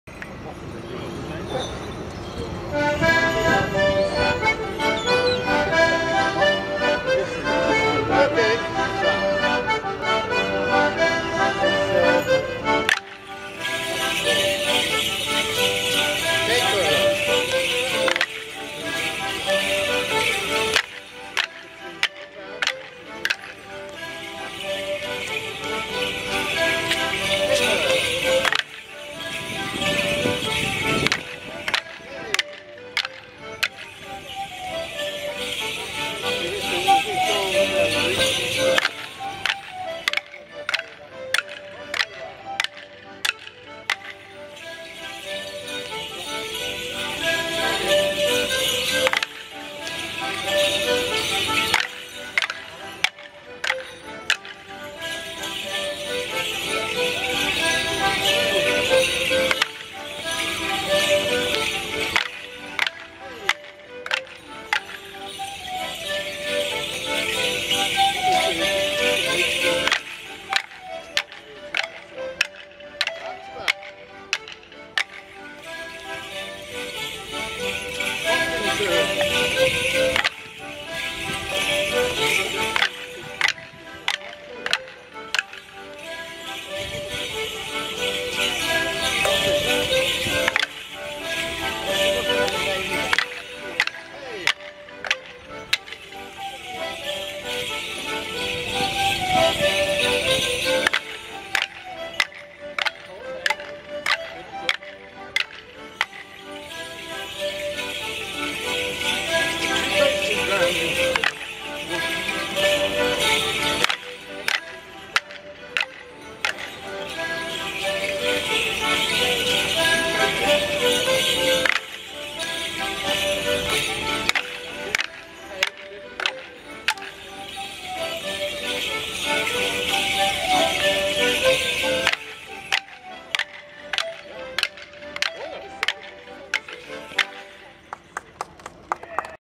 Scouse Billy, a Cotswold Morris Dance created by Mersey Morris
Mersey Morris Men dancing Scouse Billy in Liverpool on Aug 9th 2017